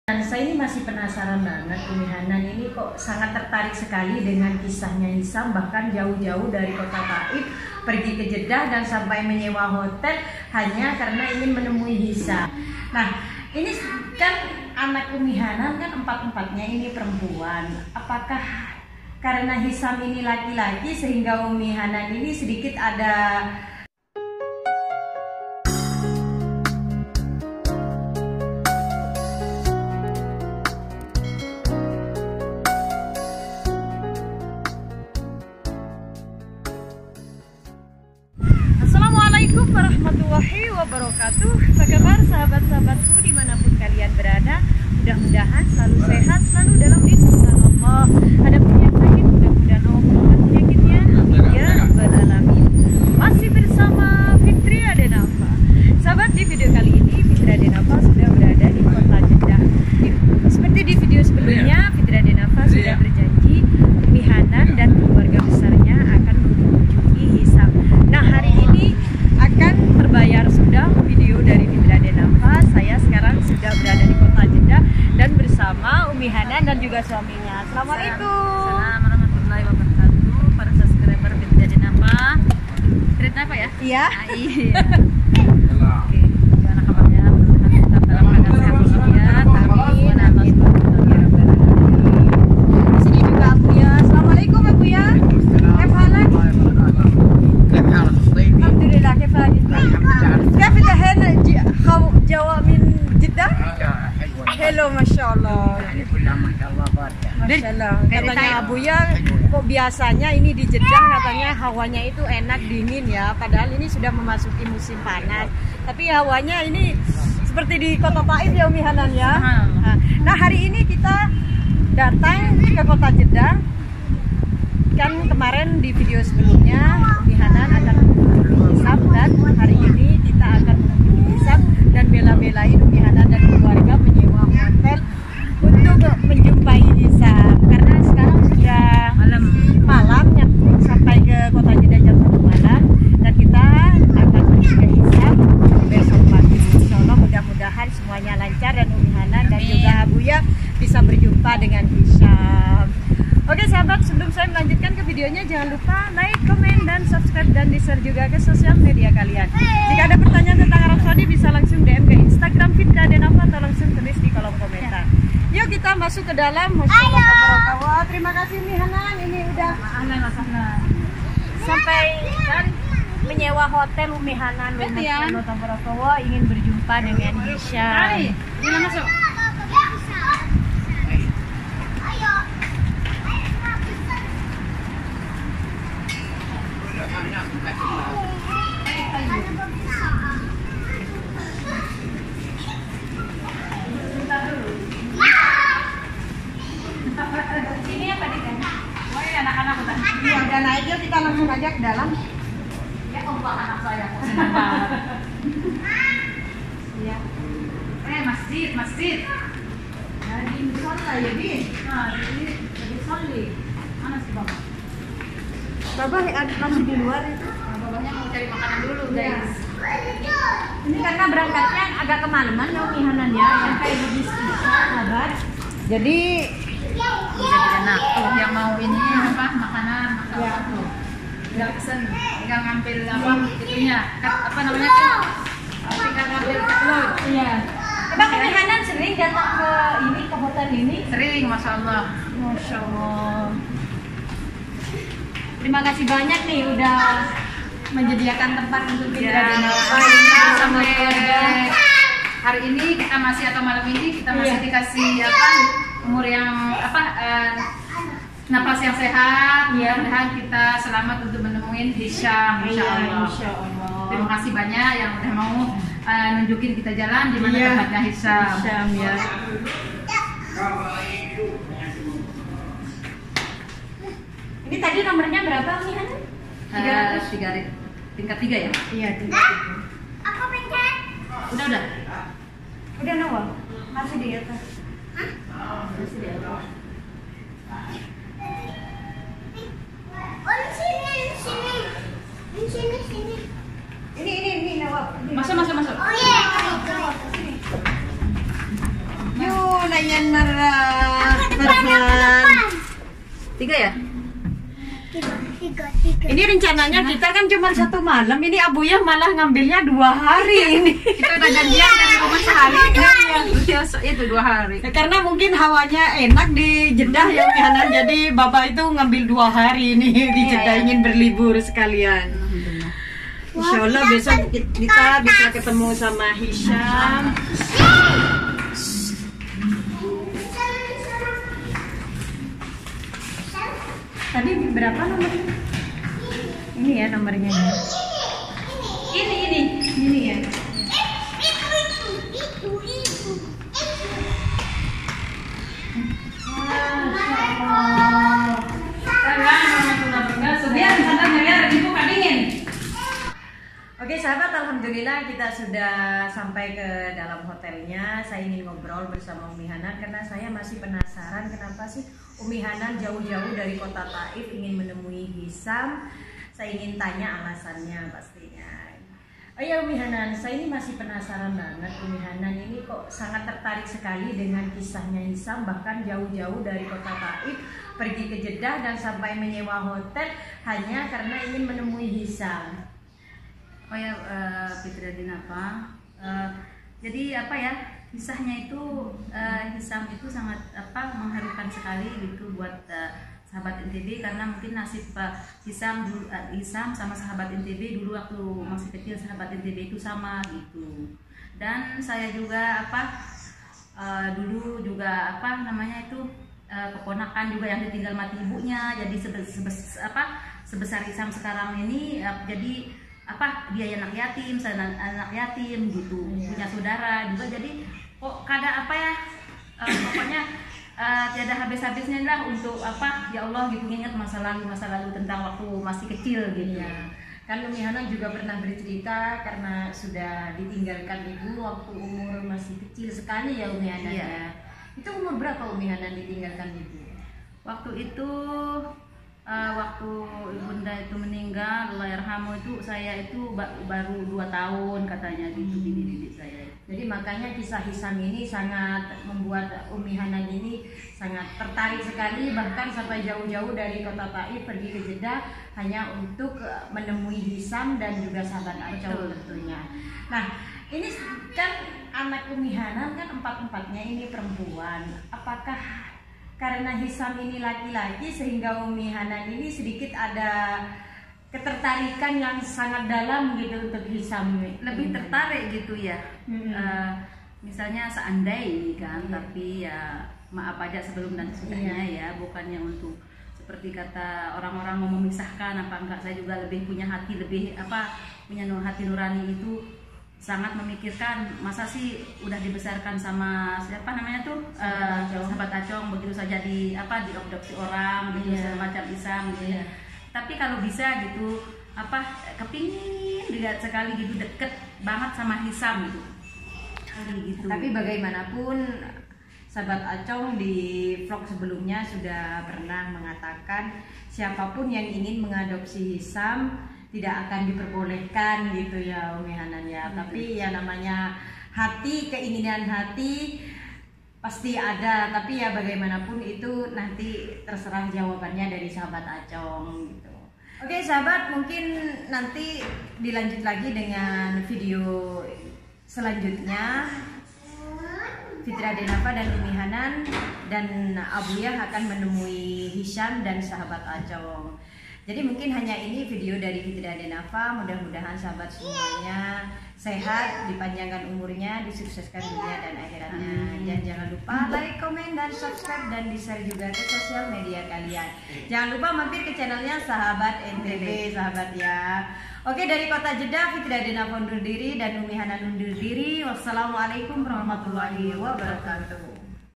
DEMI HISYAM ISTRI ORANG ARAB INI RELA MELAKUKAN INI...!!!
Assalamualaikum sahabat semuanya dimanapun kalian berada semoga selalu dalam lindungan allah aamiin

Di video ini Fitria denafa MENGANTAR istri orang Arab ini untuk mengunjungi hisyam dari kota taif menuju kota jeddah dan ini ALASAN istri orang Arab ini ingin bertemu hisyam

Semakin penasaran rasanya.!!!